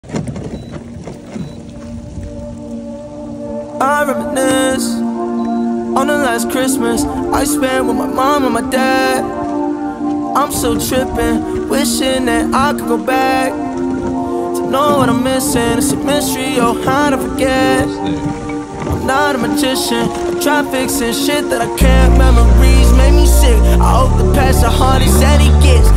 My Auto Pilot Kayak is an ICE BREAKER 💪
My Old Town AutoPilot 120 Fishing Kayak is one bad machine!  No problems punching through ice today.